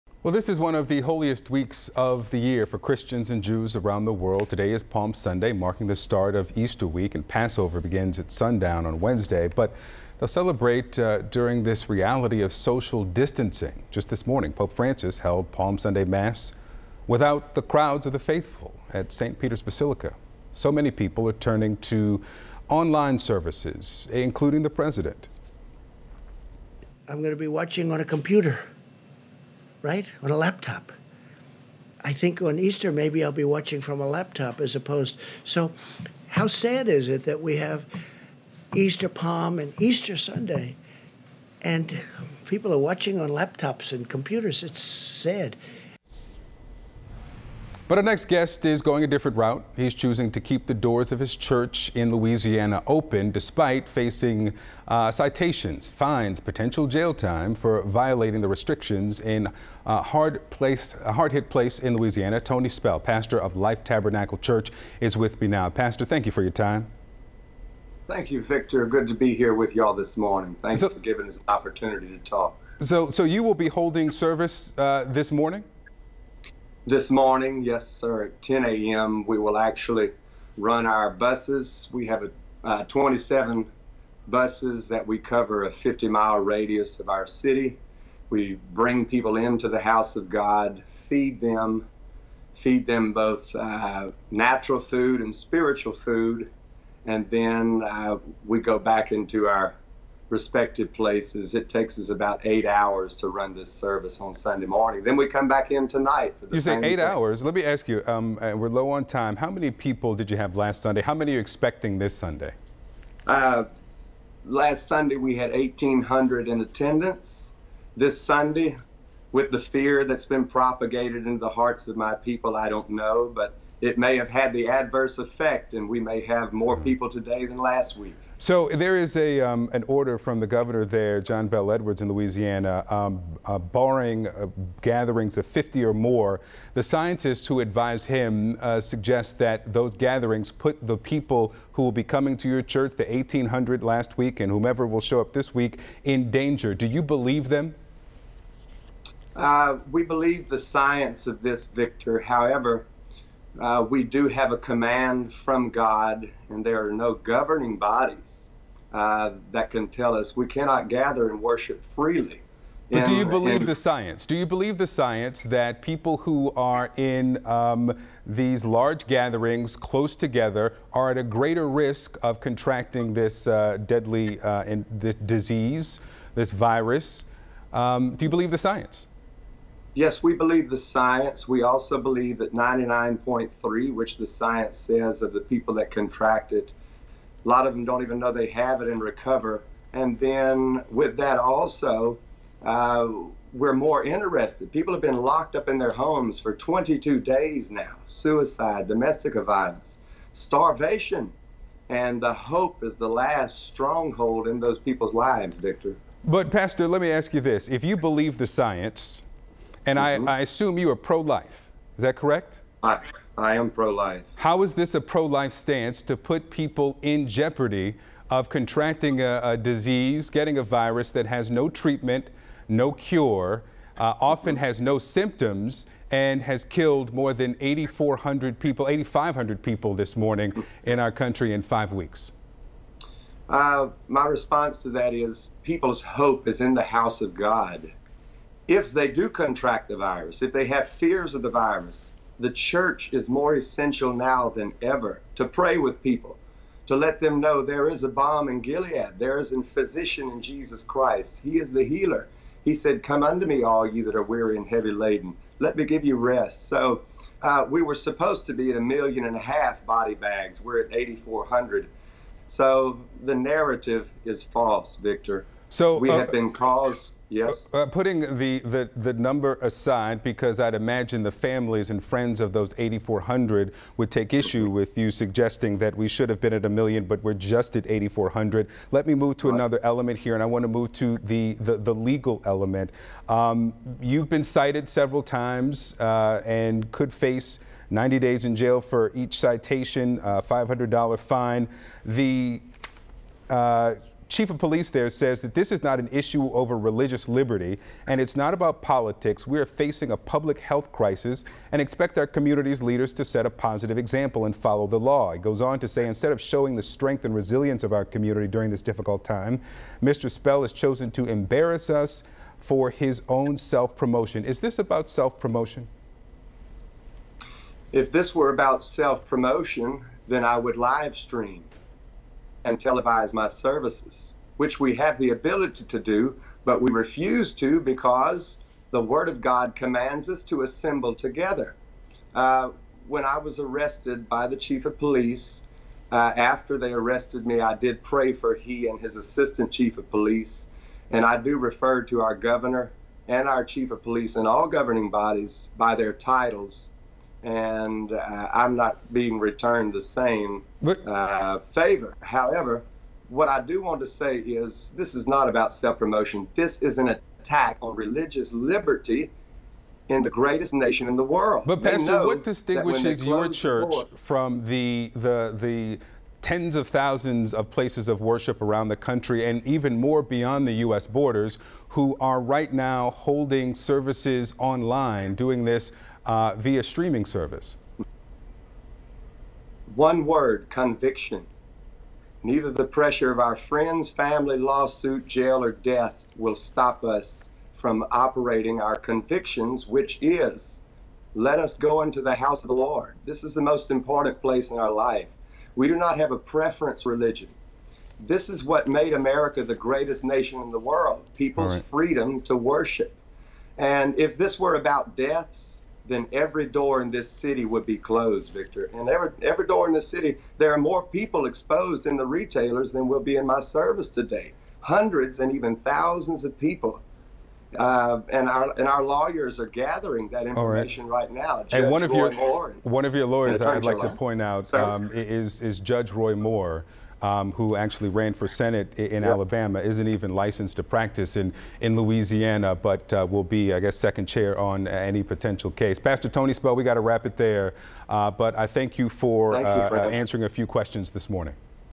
Blackwell presses pastor: How can you be pro-life and keep your church open?
CNN's Victor Blackwell speaks with Pastor Tony Spell, who has decided to keep his Louisiana church open as the coronavirus pandemic continues to spread in the US.